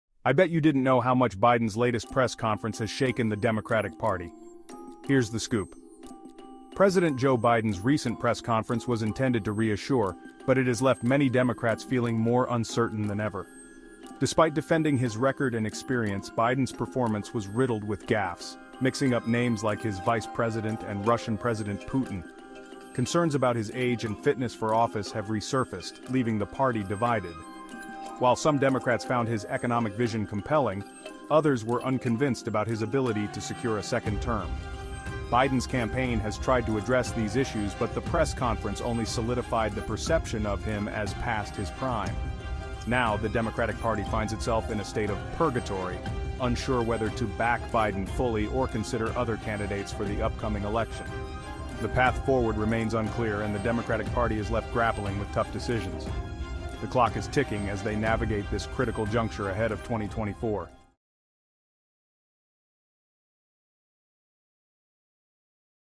Biden's Press Conference Leaves Democrats in Purgatory
In this video, we delve into the aftermath of Biden's press conference, where Democrats are left in a state of uncertainty and doubt, feeling as though they are stuck in political purgatory. Watch as we analyze the impact of this event on the Democratic party and what it could mean for the future.